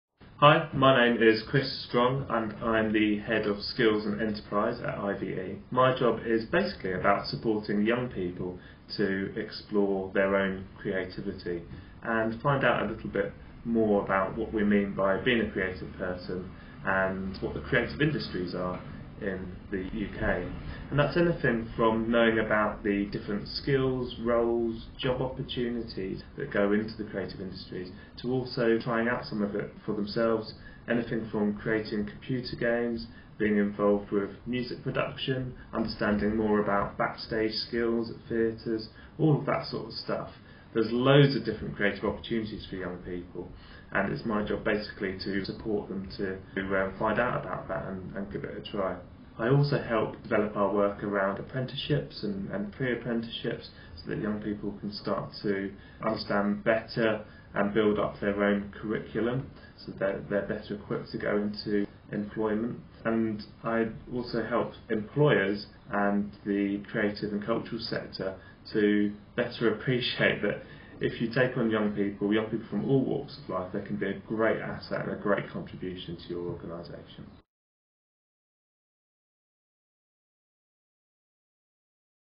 World Youth Skills Day - Shaping Creative Futures
At IVE we are working on a project which supports young people in exploring their own creativity and gaining creative skills. Watch the video to learn more or visit weareive.org